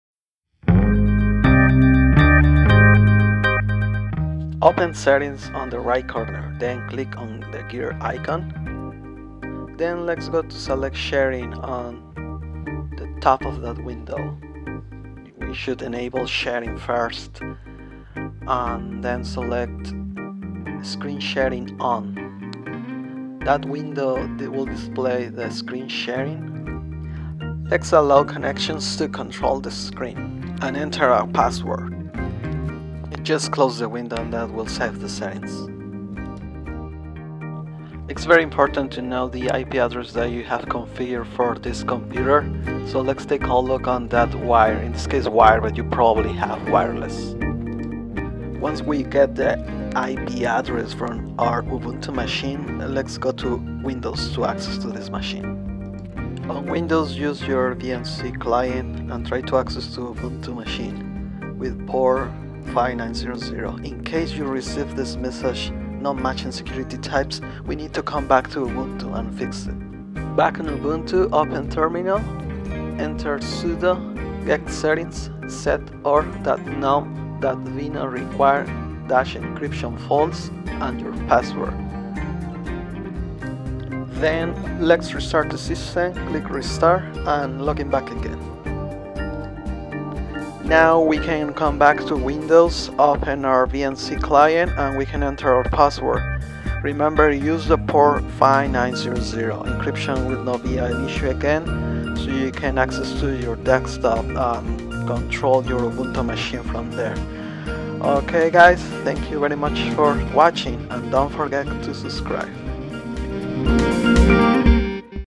How to Remote Access to Ubuntu 19.04 from Windows
How to Remote Access to Ubuntu 19.04 from Windows w/ VNC client.
RealVNC client.

Command:
~$ sudo gsettings set org.gnome.Vino require-encryption false